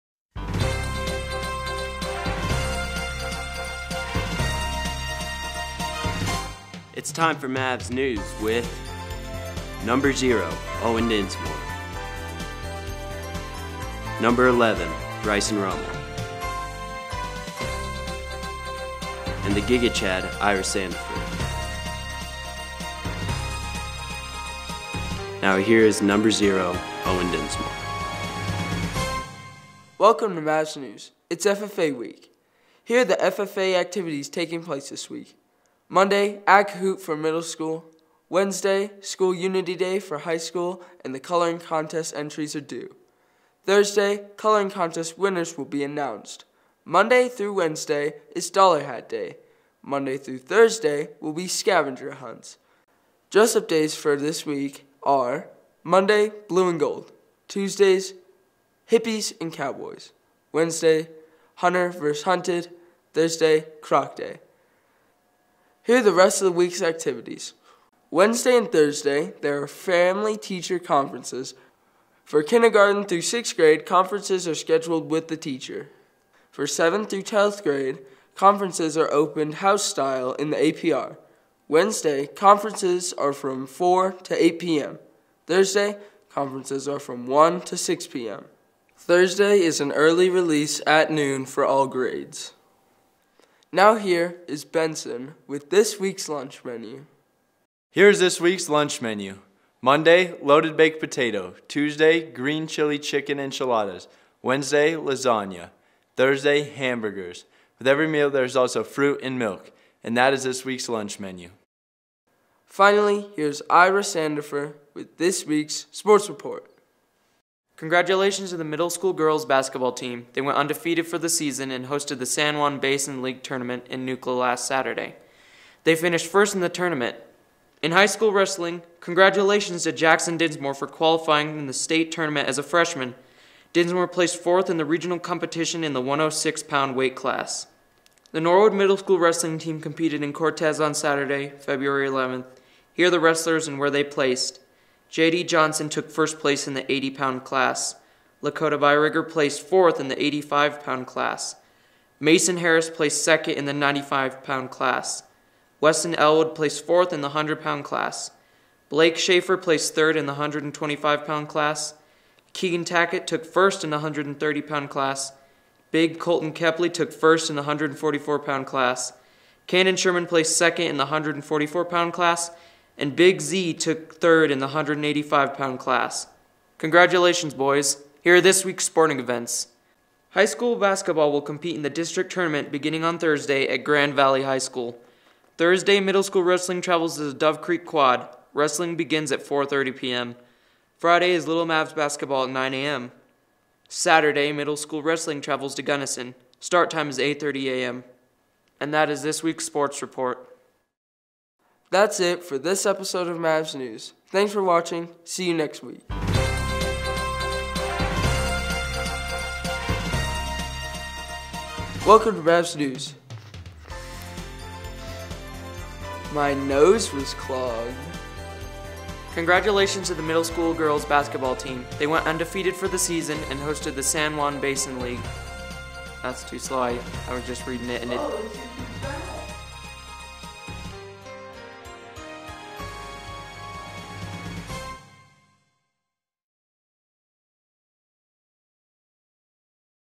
Mavs News for the Week of February 20, 2023
This is Episode 172 of Mavs News for the Week of February 20, 2023.  Owen Dinsmore anchors this week's episode.  Brycen Rummel shares the lunch menu.  Ira Sandifer has the sports report. Our announcer is Coulter Shumway.